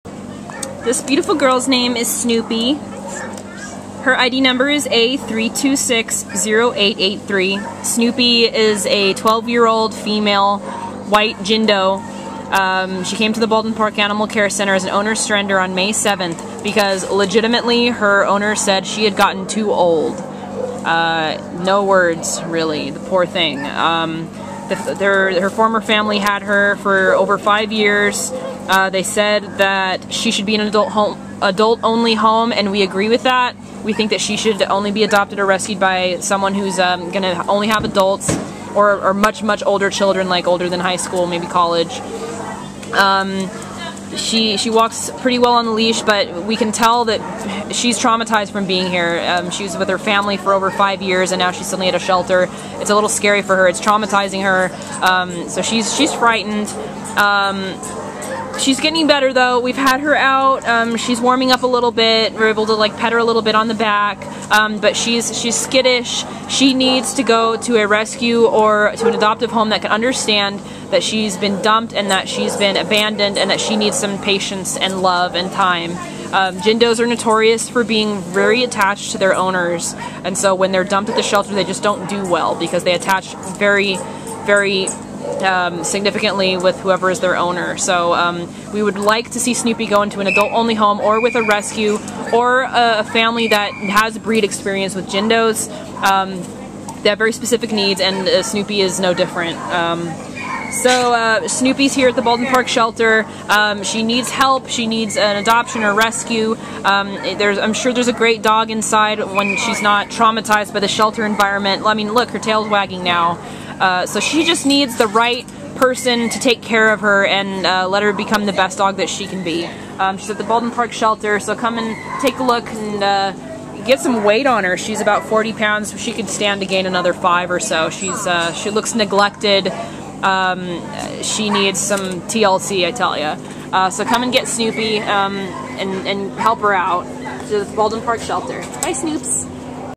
A3260883 Snoopy | Jindo
A3260883 Snoopy is a remarkable 12-year-old white female Jindo whose owners surrendered her to the Baldwin Park Animal Care Center on May 7th because they thought she had “gotten too old.” Weighing 40 lbs, Snoopy still retains lots of her youthful beauty (although her coat looks strange right now because, we think, she’s shedding her undercoat) and certainly doesn’t appear to be old. She’s very good on a leash and, according to her former owners, should be the only pet in an adult-only household. Snoopy appears to be very much out of her element in the shelter environment; she seems lost and continuously seeking the familiar. We’re assuming she’s looking for the human or humans she knows, although we can’t rule out that her age is causing some losses that lead to her impatience and confusion. We think best case is for her to escape this place either through a rescue or a person(s) experienced with the breed and wanting to give this beautiful and special girl a life worth living to make the rest of her years truly golden.